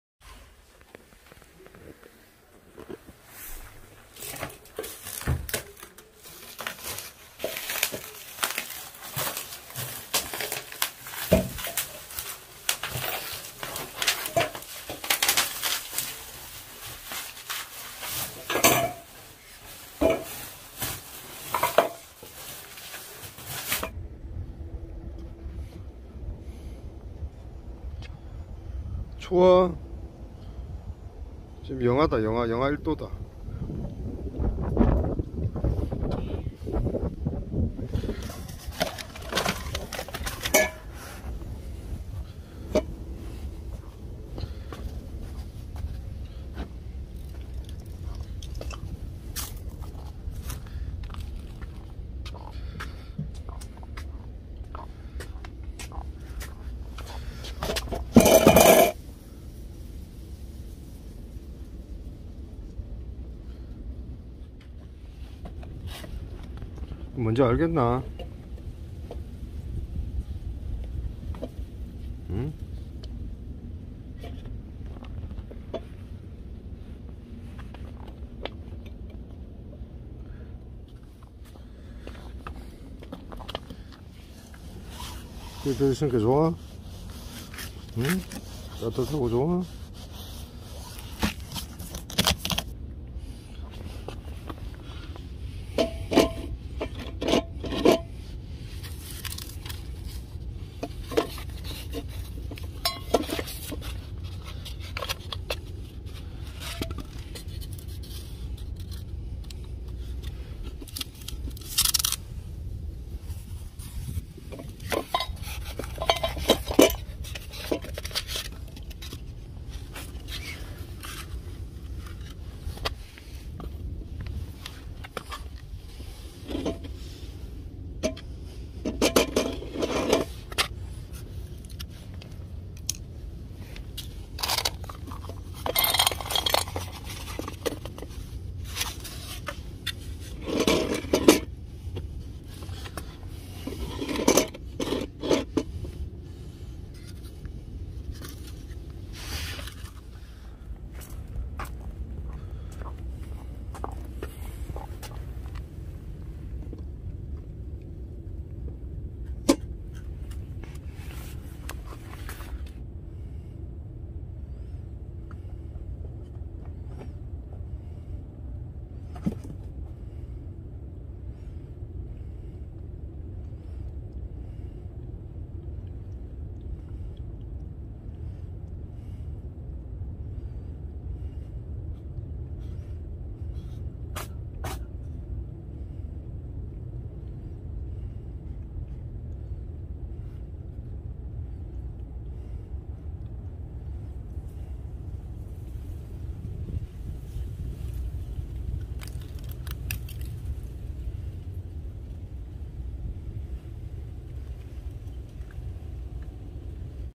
4구 밥상을 선물했습니다
빈 스티로폼 밥그릇이 찬 겨울바람에 여기저기 나뒹군다.
우리동네 자상하신 캣맘께서 늘 수고를 하시는데 조금이라도 도움이 될까 4구 밥상을 준비했다.
겨울을 나는 아이들이 조금이라도 좋은 자세로 밥을 먹었으면 좋겠다.